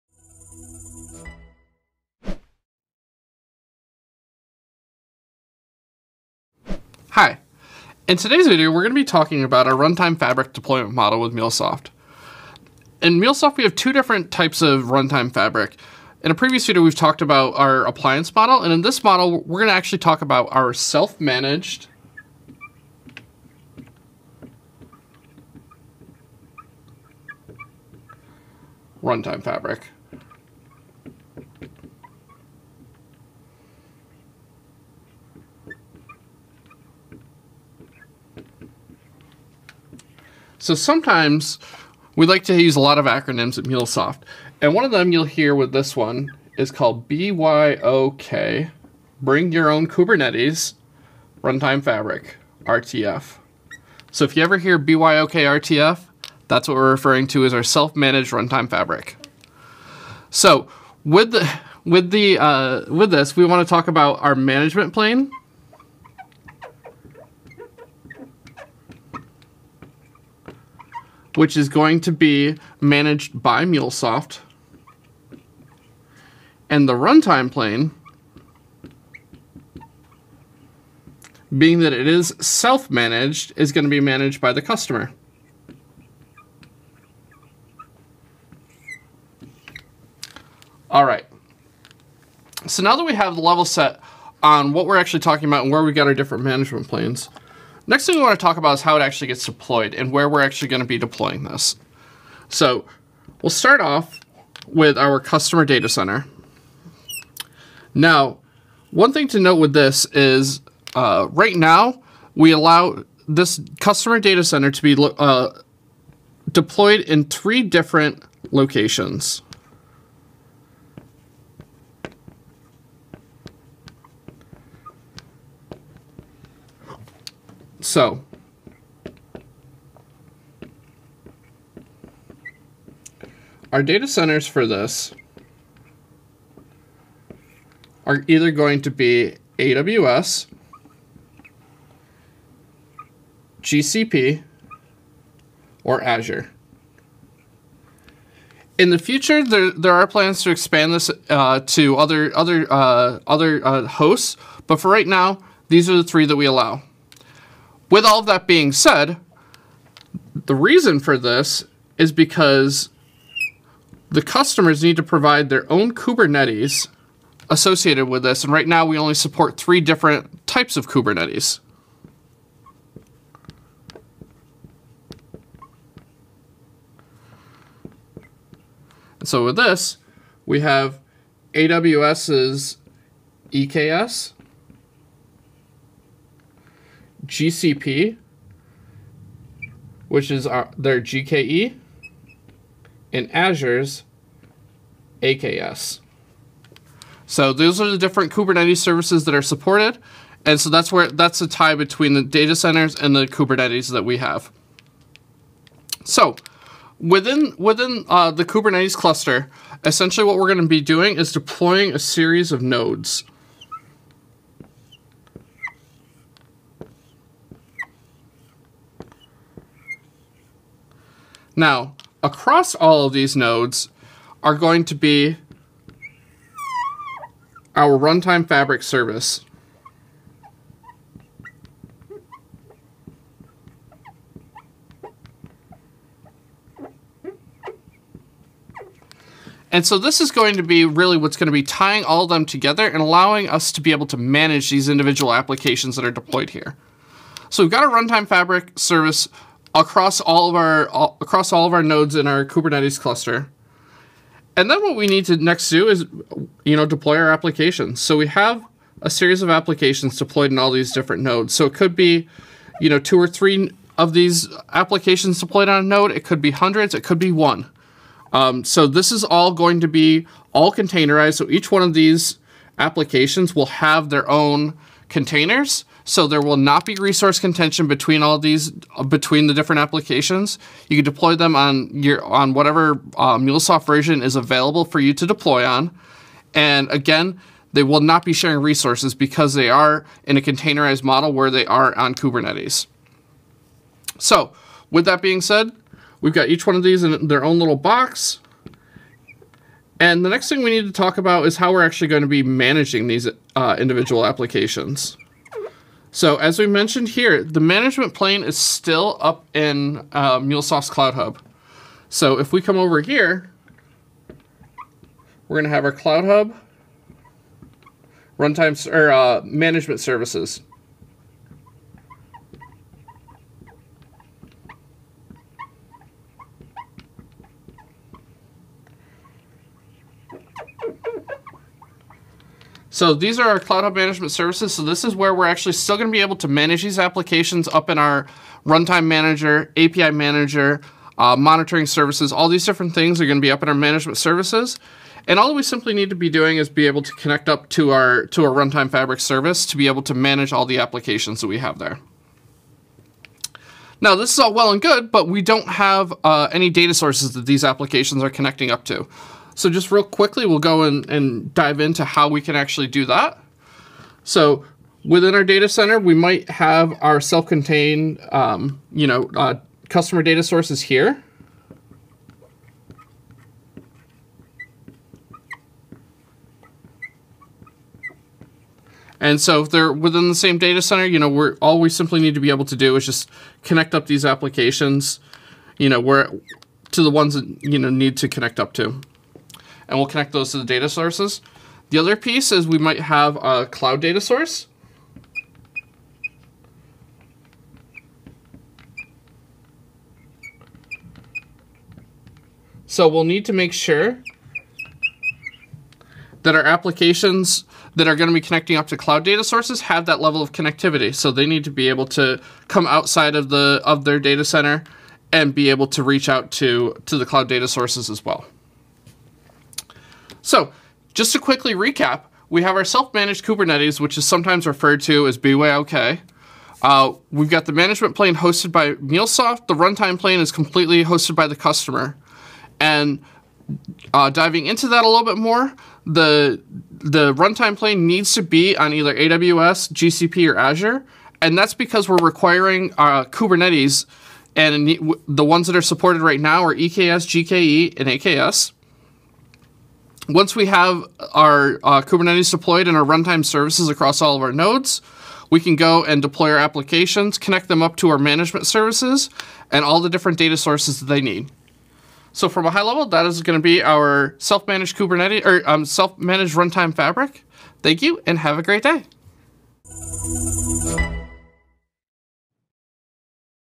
Runtime Fabric self-managed Kubernetes deployment model explained
This video discusses Runtime Fabric on self-managed Kubernetes as one of the available deployment options for the Anypoint Platform runtime plane.

Documentation for Runtime Fabric on Self-Managed Kubernetes can be found